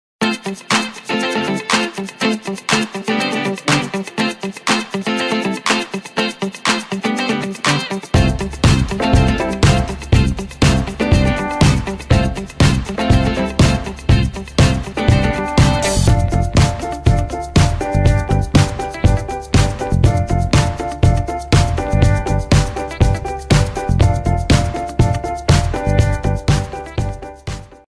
Kittens live - funny kitten compilation cute cat video
The kittens are now 4 weeks old and stronger than ever. Playing more sleeping less.